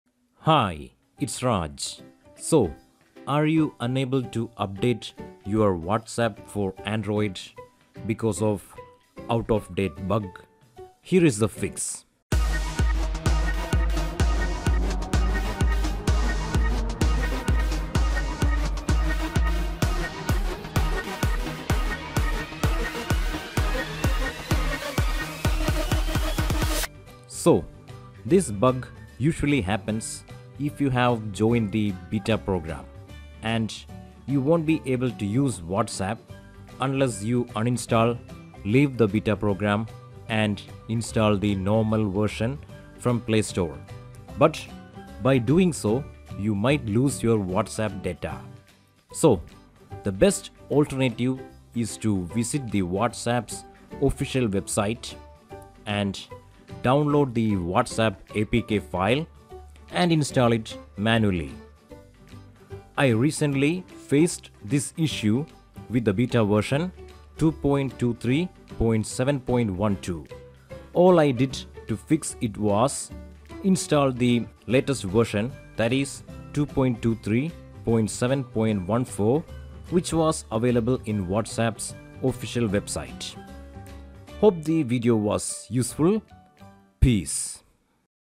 No WhatsApp Update In Google Play Store: β Out Of Date Bug - Fix
This video shows how to update WhatsApp if your version shows became out of date and no update available in play store.